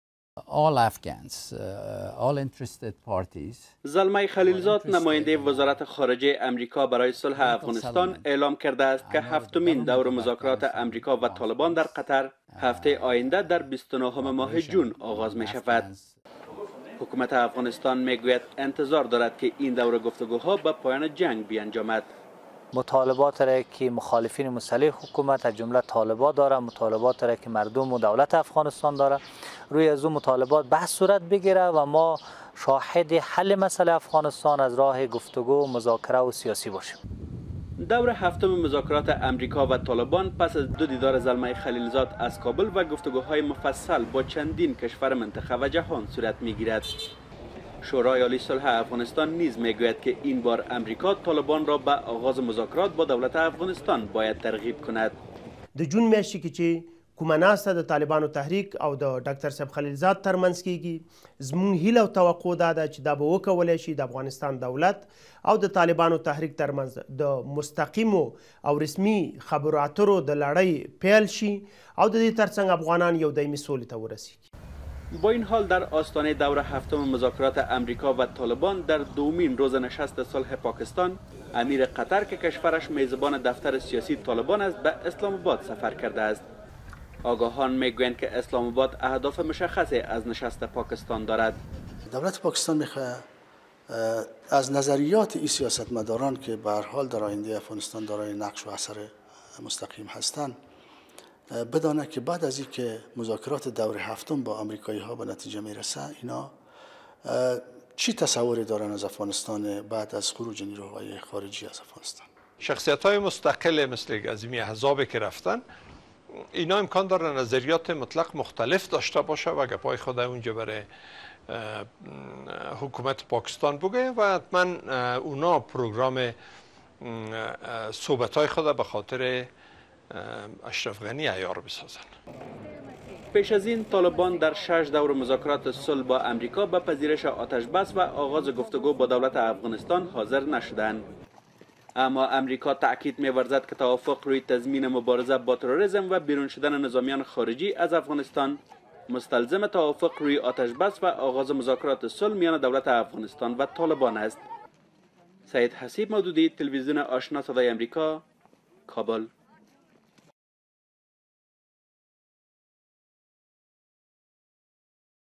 GH EA Dari FTP 7th Round of Peace Talks TRT 0240 Sec 28540937
همزمان با اعلام تأریخ دور هفتم مذاکرات امریکا و طالبان در قطر، حکومت افغانستان میگوید که انتظارِ دریافت یک راه حل سیاسی را برای ختم جنگ دارد. در همین حال، در دومین روز نشست صلح پـاکستان،‌ بـرخی از آگاهان بدین باورند که اسلام آباد می خواهد دیدگاه سیاسیون افغان را دربـارهٔ افغانستـانِ پسا خروج امریکا، دریافت کند